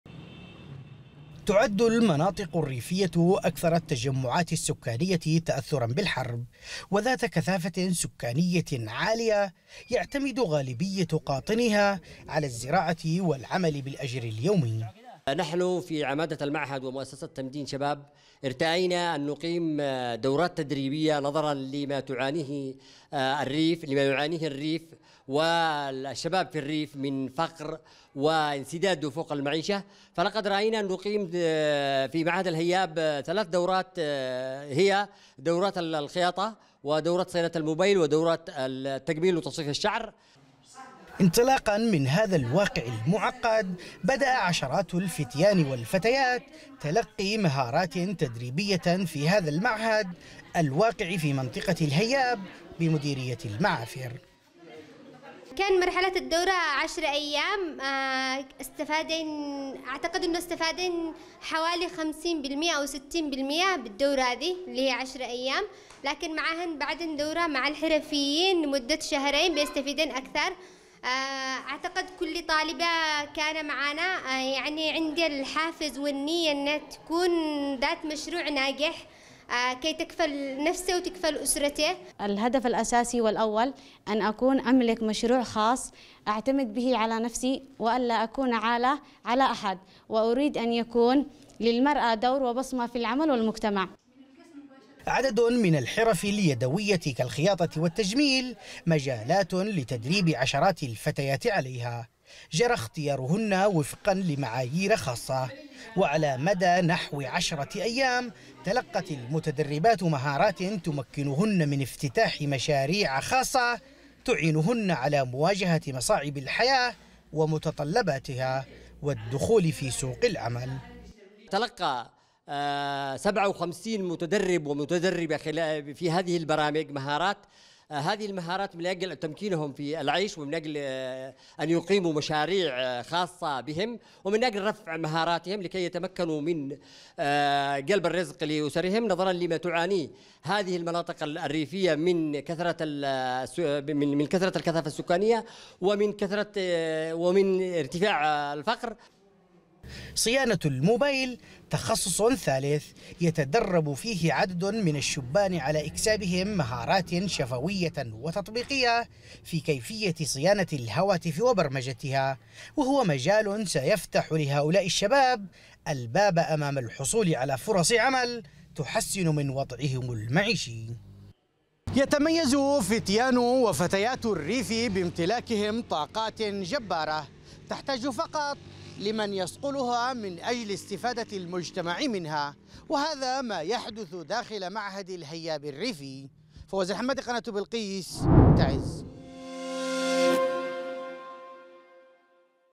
تعز .. أنشطة تدريبية ومهاراتية لـ60 شابا وفتاة بهدف تأهيلهم لسوق العمل | تقرير: فواز الحمادي
أقام معهد الهياب الريفي بمديرية المعافر في محافظة تعز دورات تدريبية لقرابة ستين متدربا ومتدربة في مجالات المهارات الحياتية.
وبحسب إدارة المعهد فإن هذه الدورات تأتي لصقل مواهب الشباب في المناطق الريفية وتأهيلهم للسوق بهدف خلق وتوفير فرص العمل لمواجهة حالة البطالة في أوساط المجتمع، وتحسين ظروفه الاقتصادية.